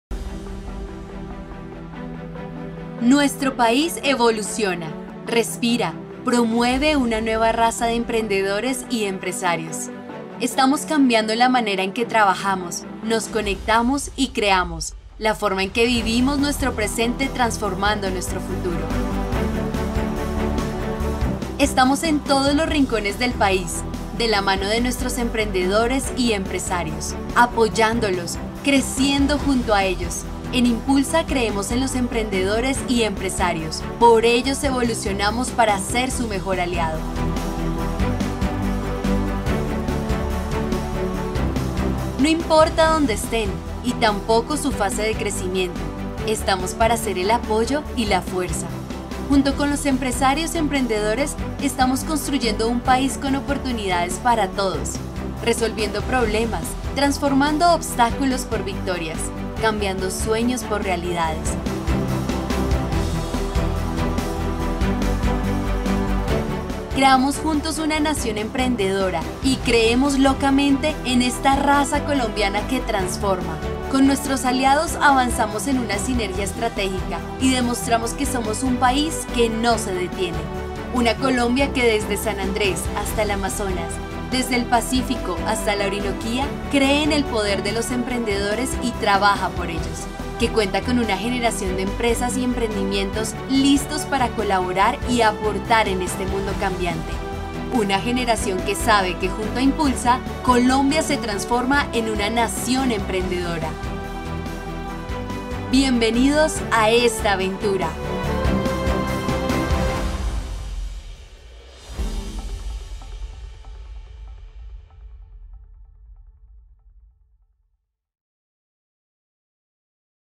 Colombia, una nación de emprendedores
¡Colombia es el tercer país más emprendedor del mundo! Por eso desde el Pacífico hasta la Orinoquía vemos una generación que construye país con oportunidades para todos. Con programas como Aldea Experimenta Avanzado, una iniciativa de iNNpulsa Colombia y la Cámara de Comercio de Cali, podrás detonar el potencial de crecimiento extraordinario de tu emprendimiento. ¡Conoce más!